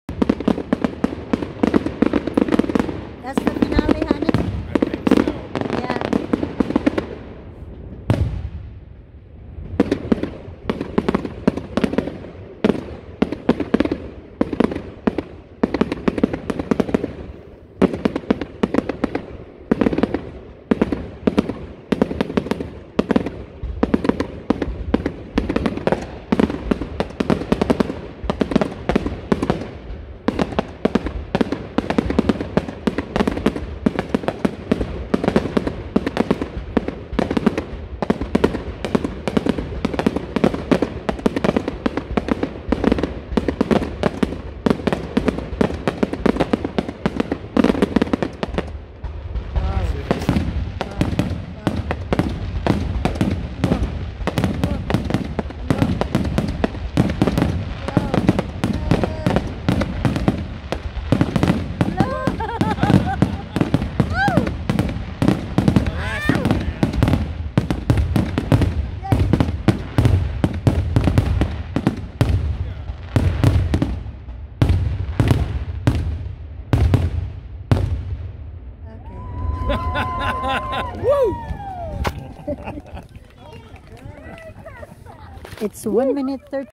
WOW! These Amazing Fireworks BROUGHT THE NEIGHBORHOOD CLOSER
Did you know that fireworks displays can bring neighborhoods closer together? In this video, we'll take a look at some of the most amazing fireworks displays in the world and show you how they bring neighborhoods together.

From Tokyo to Barcelona, we'll take a look at some of the world's most spectacular fireworks displays. Watch as these displays bring neighborhoods together and leave everyone in awe! Whether you're a fireworks lover or not, you're sure to enjoy this video!